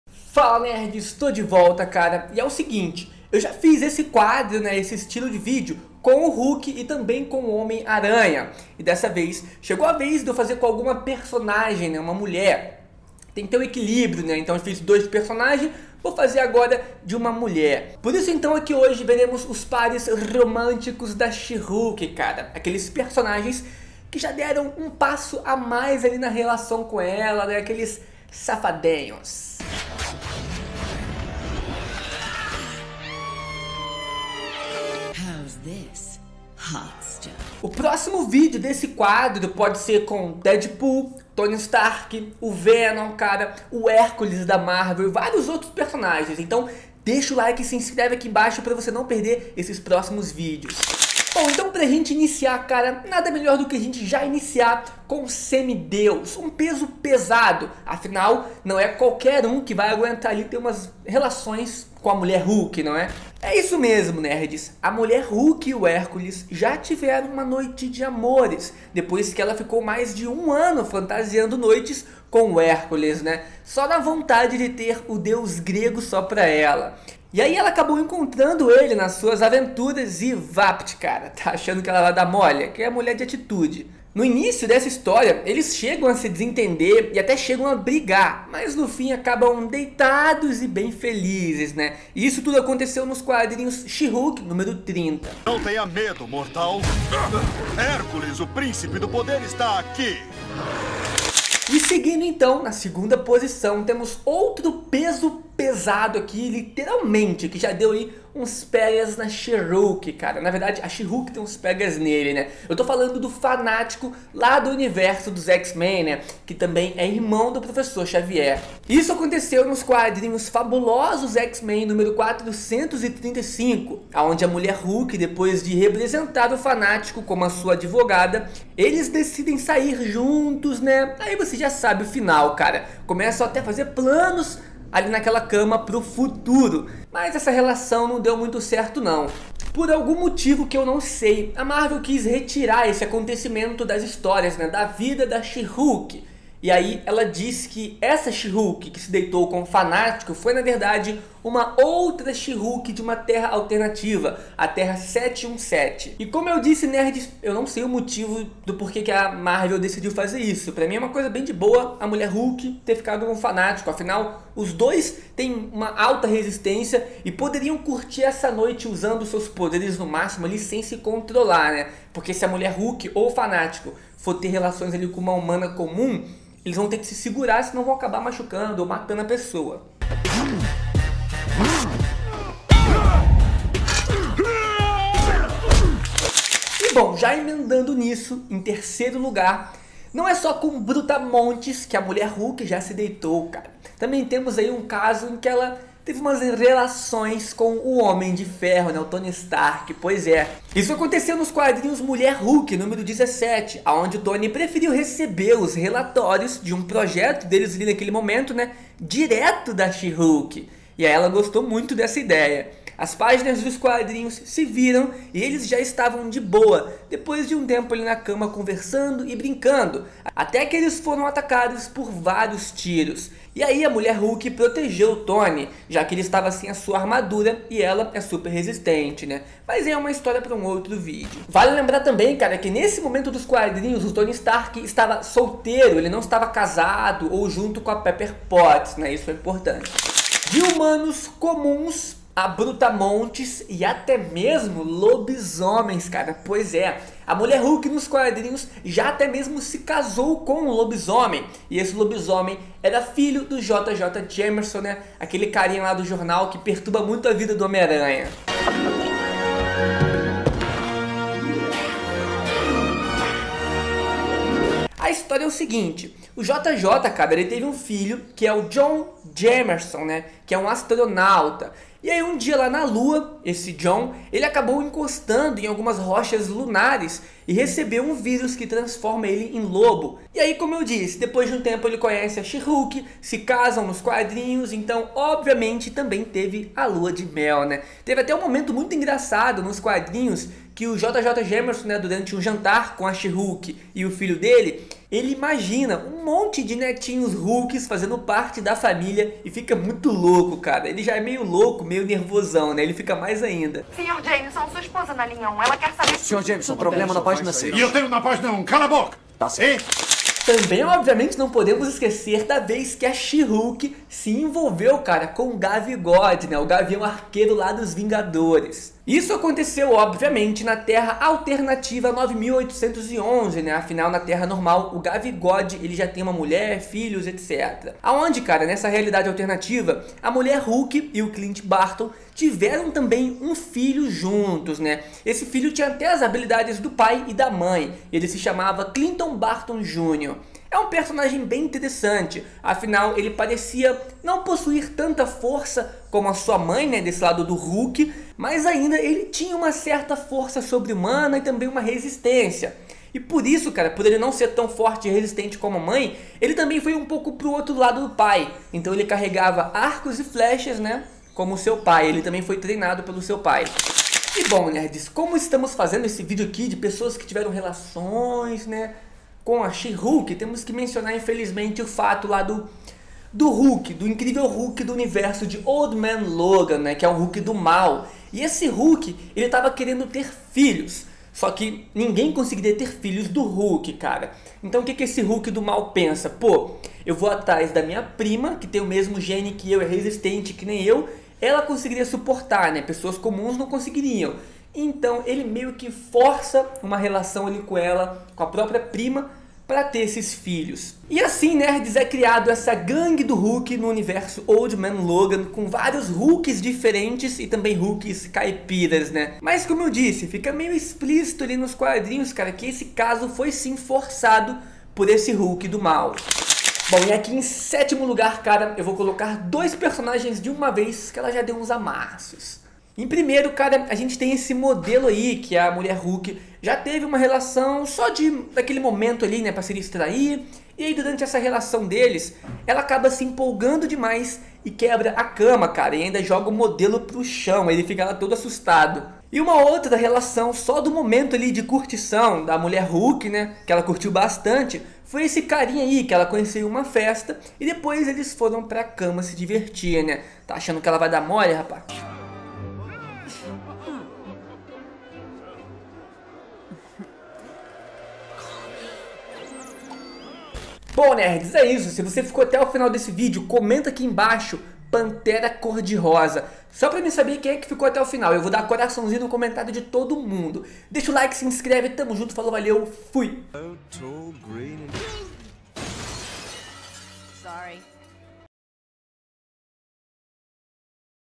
7 Personagens que já se DEITARAM com a SHE HULK
▶ Você já sabe que o hulk é sem vergonha e sai pegando todo mundo mas e se eu te falar que a prima dela a She Hulk ( Mulher Hulk ) tambem gosta de dar umas curtidas desse jeito!? psé ela já teve varios namoradinhos nos quadrinhos confere ai

Titulo: 7 Personagens que já se DEITARAM com a SHE HULK

❤ VEM CONVERSAR COMIGO!! ❤